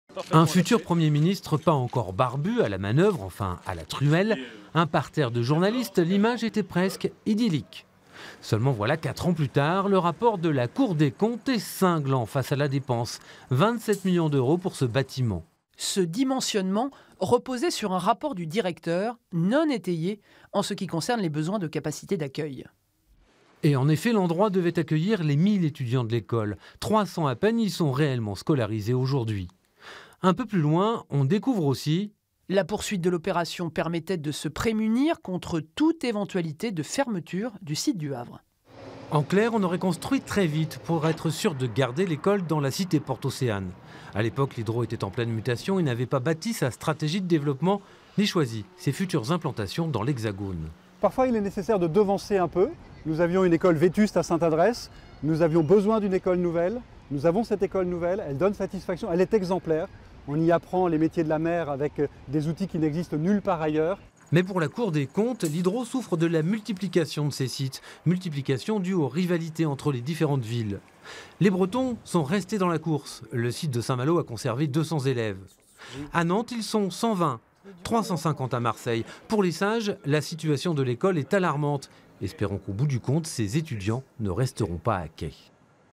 L'école de marine marchande du Havre dans le collimateur de la Cour des comptes
Un exemple de mauvaise gestion de l'argent public ?